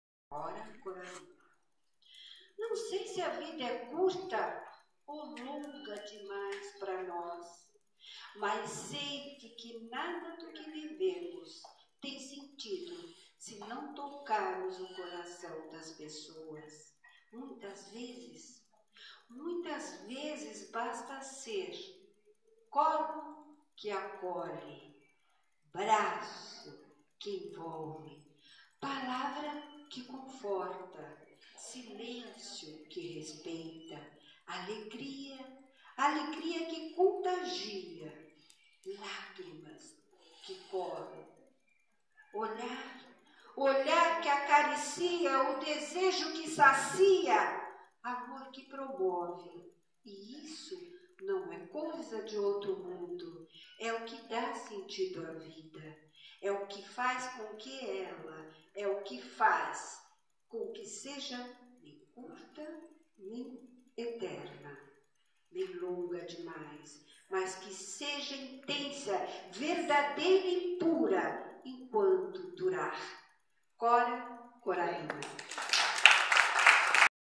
Terezinha Dias Rocha - Linda Poesia
Terezinha Dias Rocha
Biblioteca Nuto - 29/08/2014
"Grupo de Poetas, Cantores e Declamadores Independentes Voluntários de São Paulo"